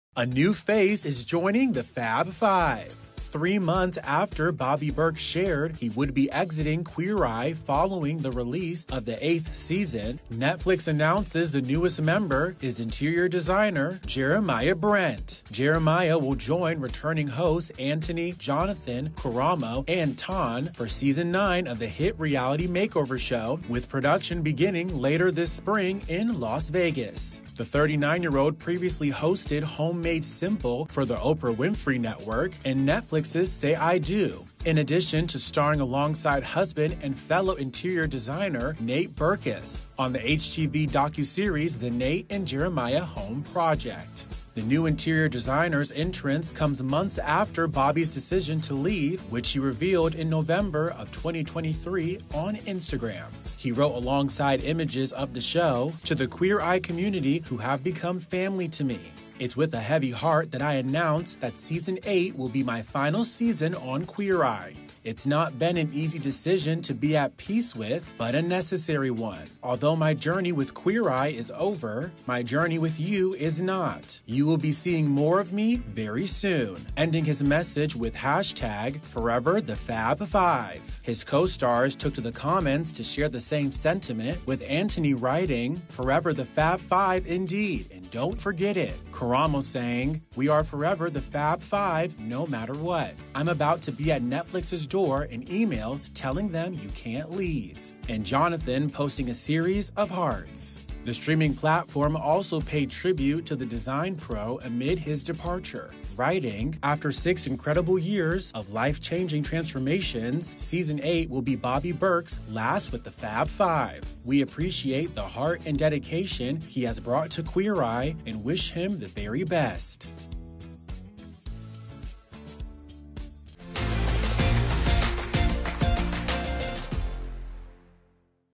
Bobby Berk’s Queer Eye Replacement REVEALED | E! News
Three months after Bobby Berk shared he would be exiting Queer Eye, Netflix announces the new member is interior designer Jeremiah Brent.